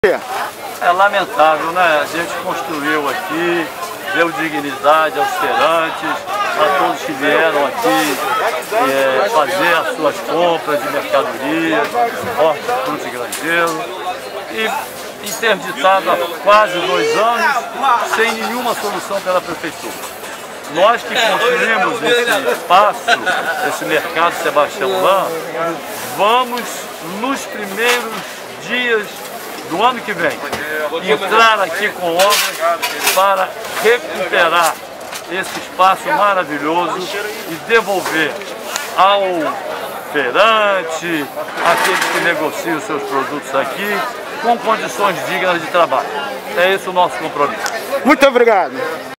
VEJA O DESOLAMENTO DE JOSÉ BONIFÁCIO COM O DESCASO DO GOVERNO COM O MERCADO SEBASTIÃO LAN
José Bonifácio fica desolado com o estado de abandono da Mercado Sebastião Lan.